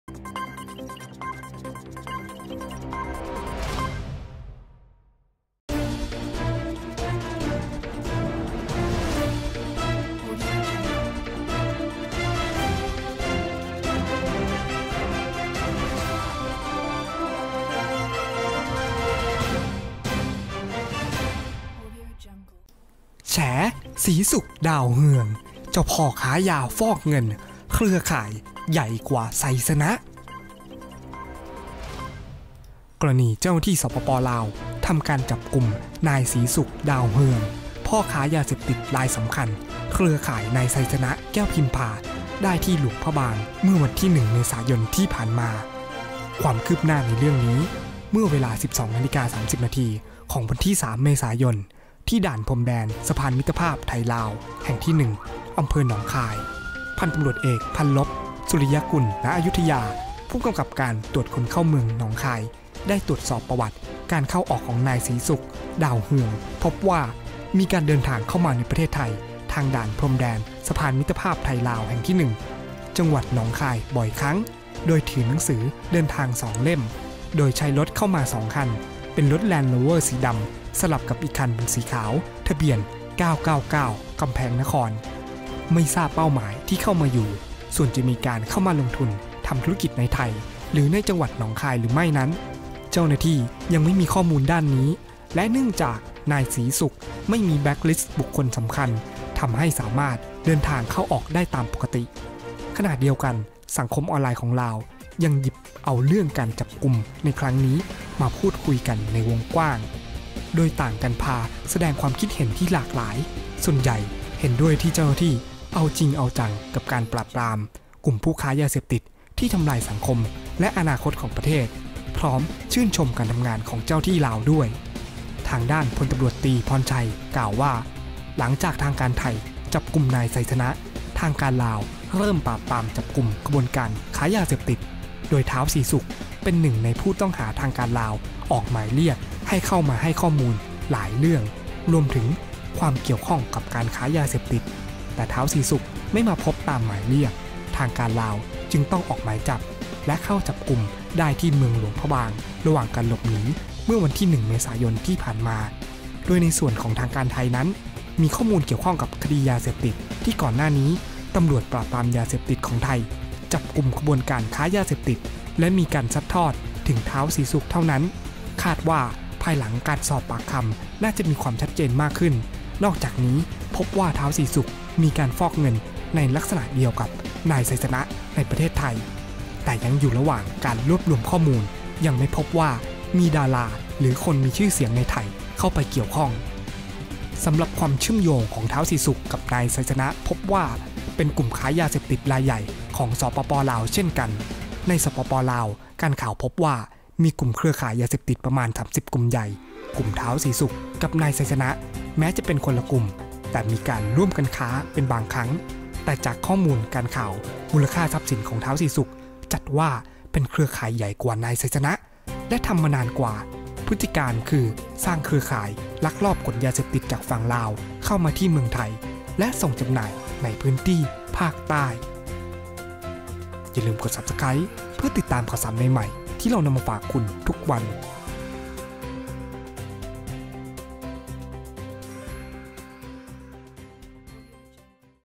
แฉ "ท้าวสีสุก ดาวเฮือง" เจ้าพ่อค้ายา ใหญ่กว่า "ไซซะนะ"
เมื่อวันที่ 3 เมษายน ตรวจคนเข้าเมืองหนองคาย เช็คประวัติพบ สีสุก ดาวเฮือง ชาวลาว เครือข่ายยาเสพติดไซซะนะ เข้าออกไทยบ่อย แต่ไม่มีแบล็คลิสต์ขึ้น ขณะที่สังคมออนไลน์ของลาวยกเป็นประเด็นร้อน ชื่นชมเจ้าหน้าที่จับกุมได้

กรณีเจ้าหน้าที่ สปป.ลาว ทำการจับกุม นายสีสุก ดาวเฮือง พ่อค้ายาเสพติดรายสำคัญ เครือข่ายนายไซซะนะแก้วพิมพา ได้ที่หลวงพระบาง สปป.ลาว เมื่อวันที่ 1 เม.ย. ที่ผ่านมา และมีรายงานว่าเจ้าหน้าที่ ปส.ของไทย จะขอเข้าสอบปากคำนายสีสุก ร่วมกับเจ้าหน้าที่ของลาวด้วยนั้น

ความคืบหน้าในเรื่องนี้ เมื่อเวลา 12.30 น. วันที่ 3 เม.ย. ที่ด่านพรมแดนสะพานมิตรภาพไทย-ลาว แห่งที่ 1 อ.เมืองหนองคาย พ.ต.อ.พัลลภ สุริยกุล ณ อยุธยา ผู้กำกับการตรวจคนเข้าเมืองหนองคาย ได้ตรวจสอบประวัติการเข้าออกของนายสีสุก ดาวเฮือง พบว่ามีการเดินทางเข้ามาในประเทศไทย ทางด่านพรมแดนสะพานมิตรภาพไทย-ลาว แห่งที่ 1 จ.หนองคาย บ่อยครั้ง โดยถือหนังสือเดินทาง 2 เล่ม เลขที่ P0881891 และ P1568042 ใช้รถเข้ามา 2 คัน เป็นรถแลนด์โรเวอร์สีดำ สลับกับ อีกคันเป็นสีขาว ทะเบียน 9999 กำแพงนคร เข้ามาทำพิธีการผ่านแดนเช่นเดียวกับคนทั่วไปตามปกติ โดยจะอยู่ในไทยประมาณ 3-10 วัน หลังจากนั้นจะเดินทางออกนอกประเทศผ่านทางสุวรรณภูมิ ไม่ทราบเป้าหมายที่เข้ามาอยู่ ส่วนจะมีการเข้ามาลงทุนทำธุรกิจในไทยหรือในจังหวัดหนองคายหรือไม่นั้นเจ้าหน้าที่ยังไม่มีข้อมูลด้านนี้ และเนื่องจากนายสีสุก ไม่มีแบล็คลิสต์บุคคลสำคัญ ทำให้สามารถเดินทางเข้าออกได้ตามปกติ

ขณะเดียวกันสังคมออนไลน์ของลาว หยิบเอาเรื่องการถูกจับในครั้งนี้มาพูดคุยกันเป็นวงกว้าง โดยนำภาพขณะที่นายสีสุก ถูกใส่กุญแจมือนั่งอยู่ในรถที่ออกอากาศทางสถานีโทรทัศน์ของไทยไปประกอบ โดยต่างพากันแสดงความคิดเห็นหลากหลาย ส่วนใหญ่เห็นด้วยที่เจ้าหน้าที่เอาจริงเอาจังกับการปราบปราม กลุ่มผู้ค้ายาเสพติดที่ทำลายสังคมและอนาคตของประเทศ พร้อมชื่นชมการทำงานของเจ้าหน้าที่ลาวด้วย

วันเดียวกัน ที่กองบัญชาการตำรวจปราบปรามยาเสพติด (บช.ปส.) พล.ต.ต.พรชัย เจริญวงษ์ รอง ผบช.ปส. เปิดเผยหลังประชุมร่วมกับ ป.ป.ส. ว่าตำรวจ บช.ปส.และ ป.ป.ส.ไทยที่ประจำการในลาว ได้ประสานขอเข้าร่วมสอบปากคำท้าวสีสุก ดาวเฮือง นักค้ายาเสพติดชาวลาว ในประเด็นเกี่ยวกับขบวนการค้ายาเสพติดในประเทศไทยที่พบหลักฐานความเชื่อมโยงกับท้าวสีสุก รวมถึงความเชื่อมโยงกับนายไซซะนะ แก้วพิมพา ผู้ต้องหาชาวลาวที่ถูกจับกุมก่อนหน้านี้ ขณะนี้รอทางการลาวยืนยันวันเข้าสอบปากคำ และทางการลาวให้ความร่วมมือเป็นอย่างดี และหลังสอบปากคำมีความเป็นไปได้ว่าอาจมีการออกหมายจับท้าวสีสุก แต่ต้องรวบรวมพยานหลักฐานให้ครบถ้วนก่อน

พล.ต.ต.พรชัย กล่าวต่อว่า หลังจากทางการไทยจับกุมนายไซซะนะ ทางการลาวเริ่มปราบปรามจับกุมขบวนการค้ายาเสพติด โดยท้าวสีสุกเป็นหนึ่งในผู้ที่ทางการลาวออกหมายเรียกเข้ามาให้ข้อมูลหลายเรื่อง รวมถึงความเกี่ยวข้องกับการค้ายาเสพติด แต่ท้าวสีสุกไม่มาพบตามหมายเรียก ทางการลาวจึงต้องออกหมายจับและเข้าจับกุมได้ที่เมืองหลวงพระบาง ระหว่างหลบหนี เมื่อ 1 เม.ย. ที่ผ่านมา

โดยในส่วนของทางการไทยนั้น มีข้อมูลเกี่ยวกับคดียาเสพติด ที่ก่อนหน้านี้ตำรวจปราบปรามยาเสพติดของไทย จับกุมขบวนการค้ายาเสพติดและมีการซัดทอดถึงท้าวสีสุกเท่านั้น คาดว่าภายหลังสอบปากคำ น่าจะมีความชัดเจนมากขึ้น นอกจากนี้ พบว่าท้าวสีสุกมีการฟอกเงินในลักษณะเดียวกับนายไซซะนะในประเทศไทย แต่ยังอยู่ระหว่างรวบรวมข้อมูล ยังไม่พบว่ามีดาราหรือคนมีชื่อเสียงในไทยเข้าไปเกี่ยวข้อง

รอง ผบช.ปส. กล่าวต่อว่า สำหรับความเชื่อมโยงของท้าวสีสุกและนายไซซะนะพบว่า เป็นกลุ่มผู้ค้ายาเสพติดรายใหญ่ของ สปป.ลาวเช่นกัน ในสปป.ลาวการข่าวพบว่ามีกลุ่มเครือข่ายยาเสพติดประมาณ 30 กลุ่มใหญ่ กลุ่มท้าวสีสุกกับนายไซซะนะ แม้จะเป็นคนละกลุ่มแต่ร่วมกันค้าเป็นบางครั้ง แต่จากข้อมูลการข่าว มูลค่าทรัพย์สินของท้าวสีสุก จัดว่าเป็นเครือข่ายใหญ่กว่านายไซซะนะ และทำมานานกว่า พฤติการณ์คือสร้างเครือข่ายลักลอบขนยาเสพติดจากฝั่งลาวเข้ามาที่เมืองไทย และส่งจำหน่ายในพื้นที่ภาคใต้

พล.ต.ต.พรชัย กล่าวอีกว่า ส่วนการจับกุมนายเทิดศักดิ์ เกตุพรม หรือหนึ่ง สำโรงเหนือ นั้น เนื่องจากขบวนการค้ายาในลาวมีมากกว่า 30 กลุ่ม และมีพฤติการณ์ค้ายาคล้ายกันคือใช้ไทยเป็นทางผ่านลงสู่ภาคใต้ จึงยังไม่ชัดเจนว่า เป็นเครือข่ายเดียวกับนายไซซะนะ และท้าวสีสุกหรือไม่ เจ้าหน้าที่อยู่ระหว่างการตรวจสอบทั้งเส้นทางยาเสพติด และเส้นทางการเงิน